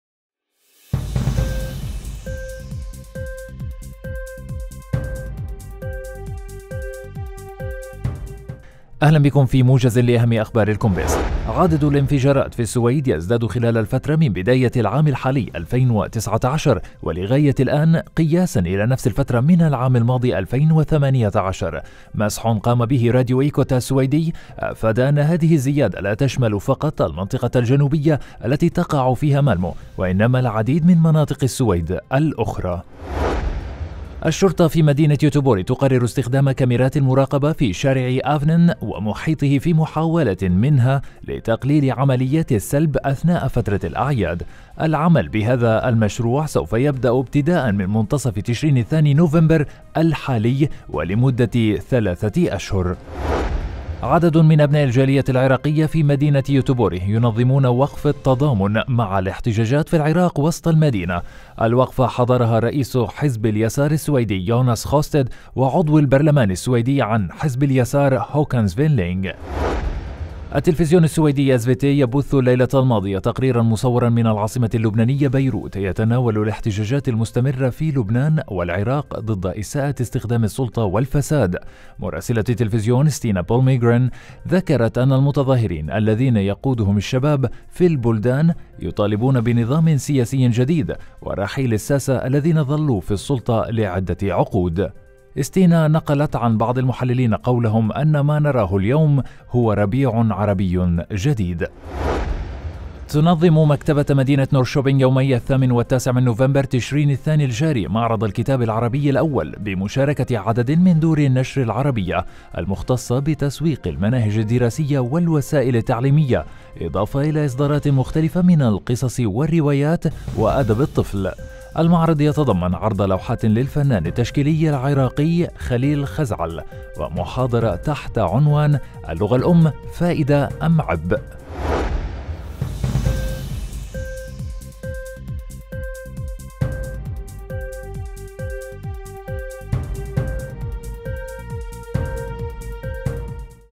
مسح يُظهر زيادة في عدد الانفجارات التي وقعت في العام 2019 مقارنة بالعام الماضي
شاهد موجز أخبار الكومبس: مسح يُظهر زيادة في عدد الانفجارات التي وقعت في العام 2019 مقارنة بالعام الماضي .. والتلفزيون السويدي يبث تقارير ميدانية من لبنان والعراق حول الاحتجاجات المستمرة في البلدين